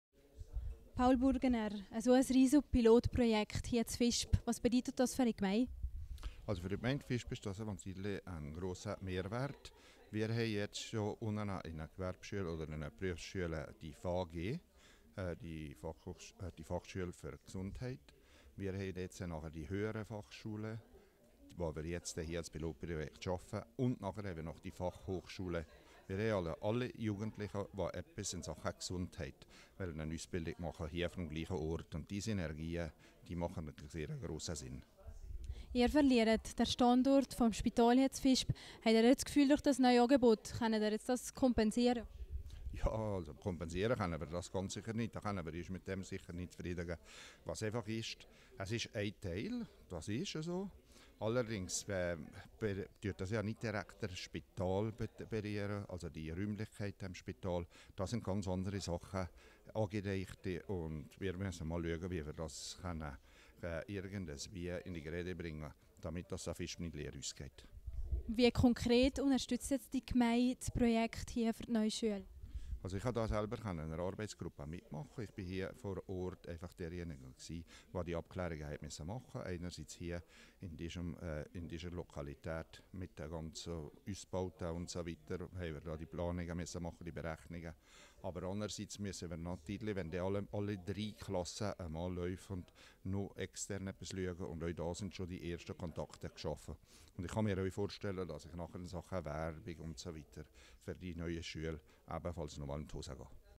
Gemeinderat und Mitarbeiter der Arbeitsgruppe Paul Burgener
Paul Burgener zum neuen Bildungsangebot in Visp.